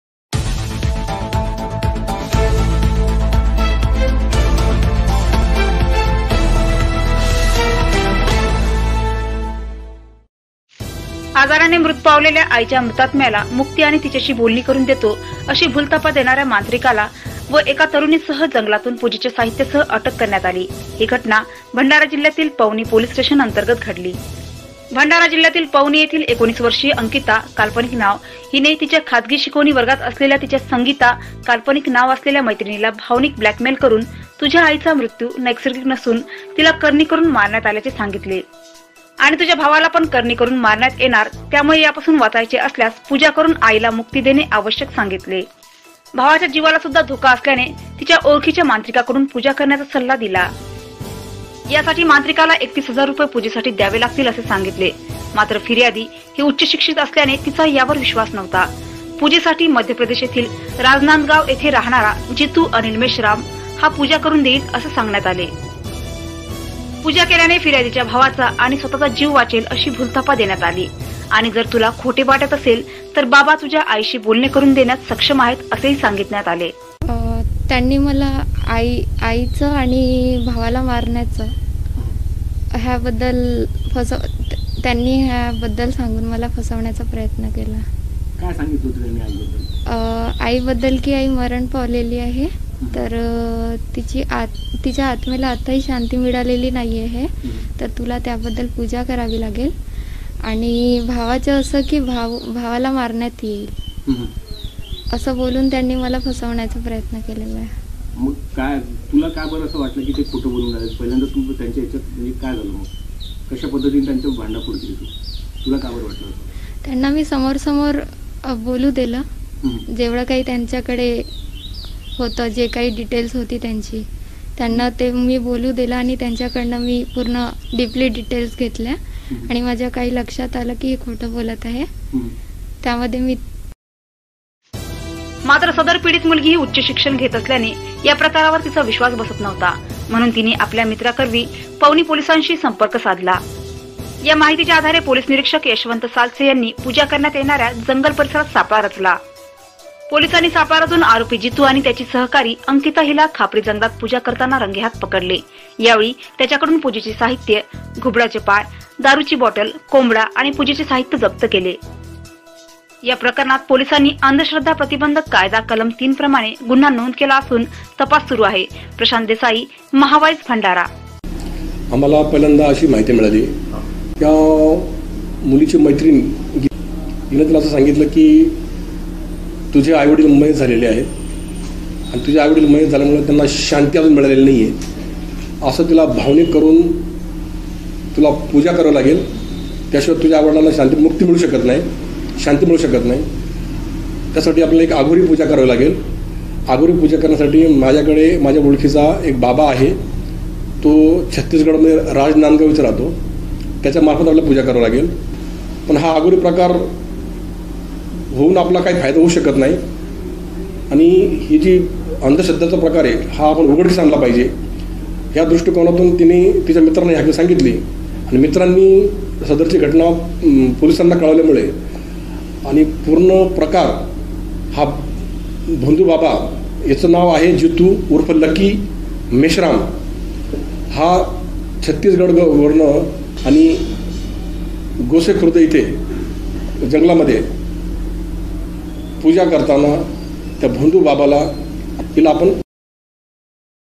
मृतात्म्याला मुक्ती मिळवून देण्याच्या नावावर भोंदूगिरी करणाऱ्या मंत्रिकास अटक
मृतात्म्याला मुक्ती मिळवून देण्याच्या नावावर भोंदूगिरी करणाऱ्या मंत्रिकास युवतीसह अटक

उच्च शिक्षित तरुणीने केला भांडाफोड

पवनी पोलिसांची कारवाई

Anchor आजाराने मृत पावलेल्या आईच्या मृतात्म्याला मुक्ती आणि तिच्याशी बोलणी करून देतो, अशी भूलथापा देणाऱ्या मांत्रिकाला एका तरुणीसह जंगलातून पूजेच्या साहित्यासह अटक करण्यात आली. ही घटना भंडारा जिल्ह्यातील पवनी पोलीस स्टेशन अंतर्गरत घडली.

Vivo  -  भंडारा जिल्ह्यातील पवनी येथील एकोणविस वर्षीय अंकिता ( काल्पनिक नाव ) हिने तिच्या खासगी शिकवणी वर्गात असलेल्या तिच्या संगीता  ( काल्पनिक नाव ) मैत्रिणीला भावनिक ब्लॅकमेल करून तुझ्या आईचा मृत्यू नैसर्गिक नसून तिला करणी करून मारण्यात आल्याचे सांगितले. आणि तुझ्या भावाला पण करणी करून मारण्यात येणार त्यामुळे यापासून वाचायचे असल्यास पूजा करून आईला मुक्ती देणे आवश्यक सांगितले. सोबतच भावाच्या जीवळसुद्धा धोखा असल्याने तिच्या ओळखीच्या मंत्रिकाकडून पूजा करण्याचा सल्ला दिला. यासाठी मांत्रिकाला एकत्तीस हजार रुपये पूजेसाठी द्यावे लागेल असे सांगितले. मात्र फिर्यादी ही उच्च शिक्षित असल्याने तिचा यावर विश्वास नव्हता. पूजेसाठी मध्यप्रदेश येथील राजनांदगाव येथे राहणारा जितू अनिल मेश्राम याच्या हा पूजा करून देईल, असे सांगण्यात आले. पूजा केल्याने फिर्यादीच्या भावाचा आणि स्वतःचा जीव वाचेल अशी भूलथापा देण्यात आली. आणि जर तुला खोटे वाटत असेल तर बाबा तुझ्या आईशी बोलणे करून देण्यास सक्षम आहे असेही सांगण्यात आले.

बाईट -    फिर्यादी तरुणी

               मात्र सदर पीडित मुलगी  हि उच्च शिक्षण घेत असल्याने या प्रकारावर तिचा विश्वास बसत नव्हता. म्हणून तिने आपल्या मित्रांकरवी पवनी पोलिसांशी संपर्क साधला. या माहितीच्या आधारे पोलीस निरीक्षण यशवंत सालसे यांनी पूजा करण्यात येणाऱ्या जंगल परिसरात सापळा रचला. पोलिसांनी सापळा रचून आरोपी जितू आणि त्याची सहकारी अंकिता हिला खापरी जंगलात पूजा करताना रंगेहात पकडले. यावेळी त्यांचे कडून पूजेचे साहित्य, घुबडाचे पाय, दारूची बॉटल, कोंबडा आणि पूजेचे साहित्य जप्त केले. या प्रकरणात पोलिसांनी अंधश्रद्धा प्रतिबंधक कायदा कलम तीन प्रमाणे गुन्हा नोंद केला असून तपस सुरु आहे.

बाईट -   यशवंत सोलसे  PI  पवनी

 प्रशांत देसाई महाव्हाईस भंडारा